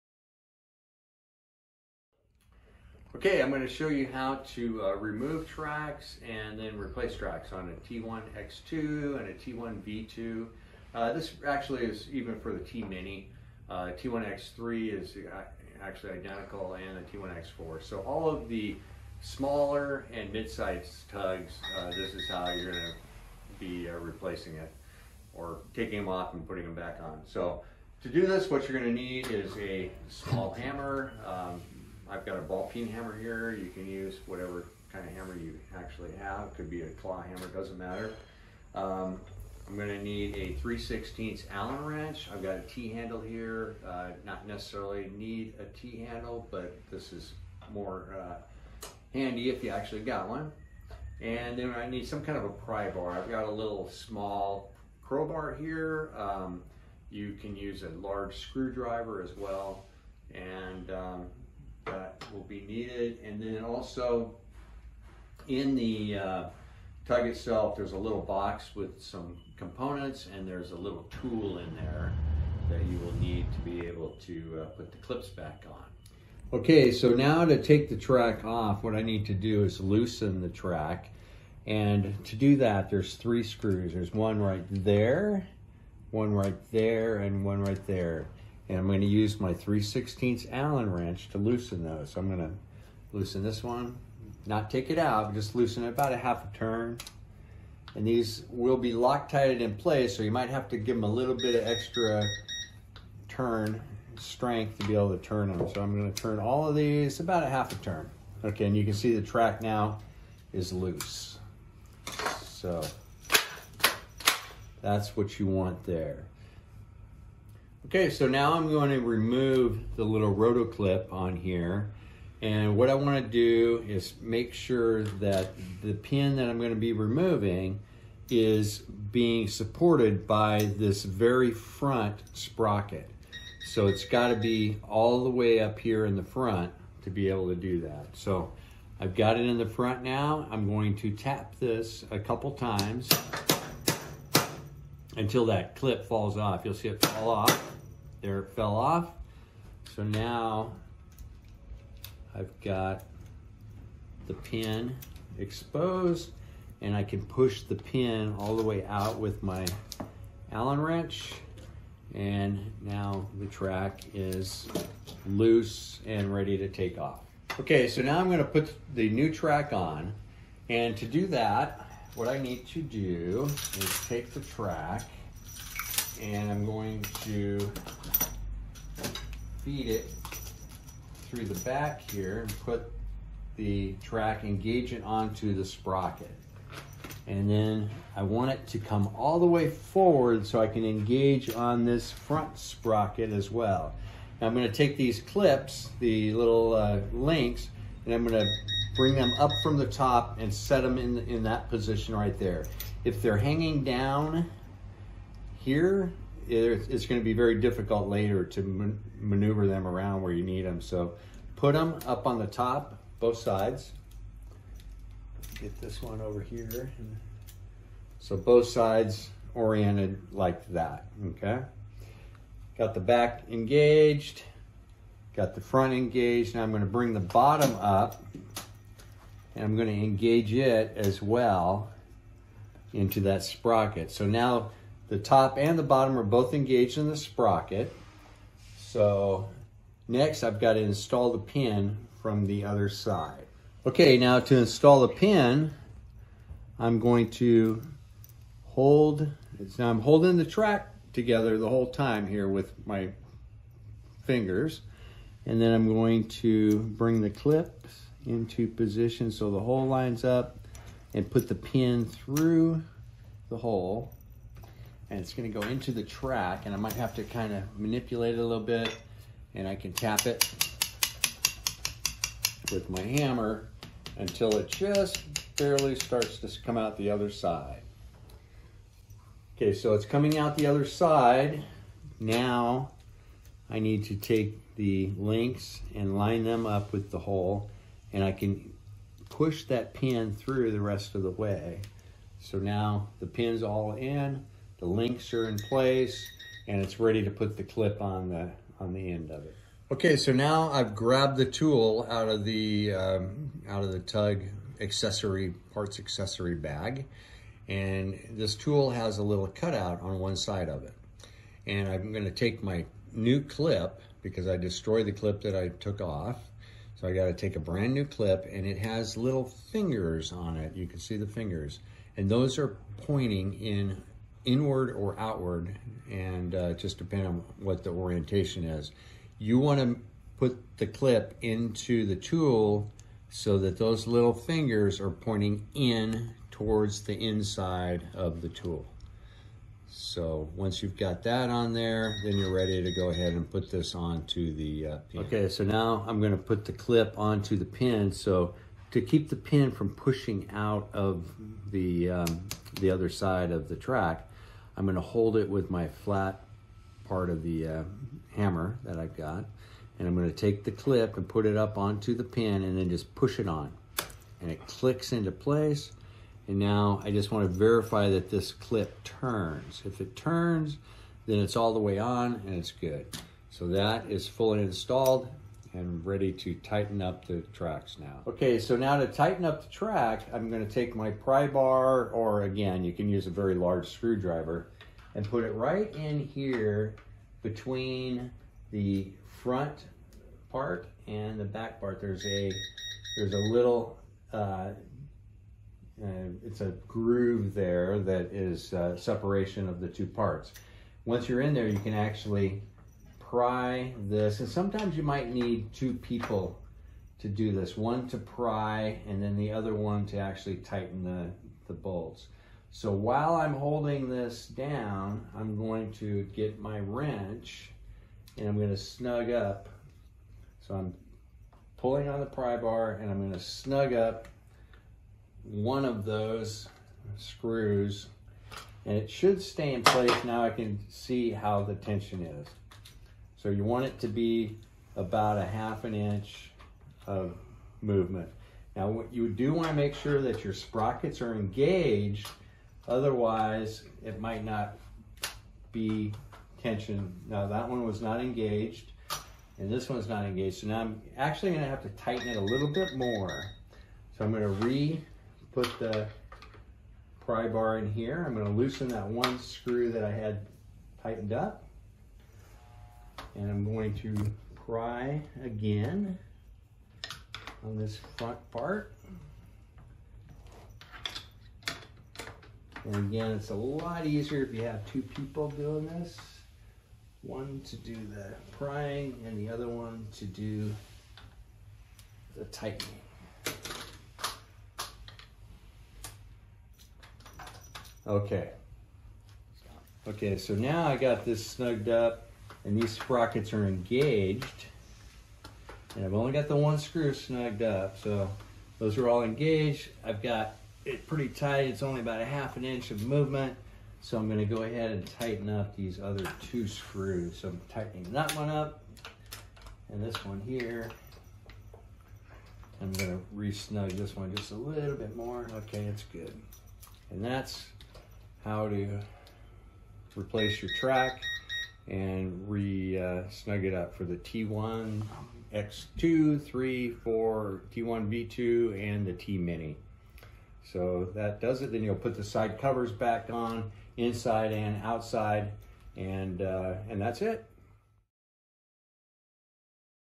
T1X2/T1X3/T1X4/T1V2 Track Replacement
Welcome to the cutting-edge world of aircraft tug upgrades! In this video, we explore the transformative impact of T1X3 Track Replacement on your aircraft tug's performance. Upgrade your tug's capabilities, enhance precision, and experience a new level of power. 🚀

Explore the features and benefits of T1X3 Track Replacement with real-life demonstrations and expert insights. From increased efficiency to unmatched durability, discover why this upgrade is a game-changer for REMOTE CONTROL AIRCRAFT TUGS.

Don't miss out on the chance to revolutionize your tug operations. Watch the video now and take the first step toward unlocking the full potential of your aircraft tug!